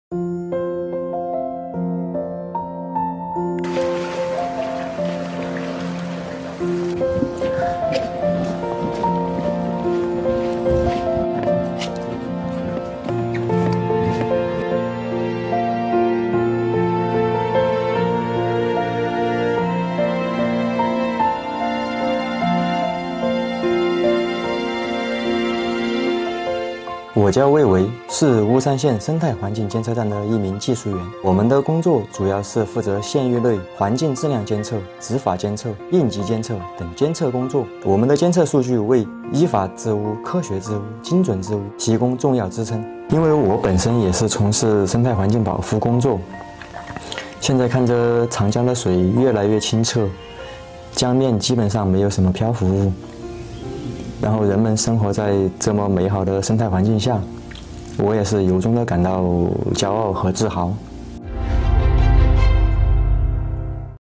GLOBALink | Safeguarding water quality of Yangtze River
After graduating from college in 2005, Wei Wei has been devoting himself to safeguarding the water quality of the Yangtze River in China's Chongqing. He feels proud of his work.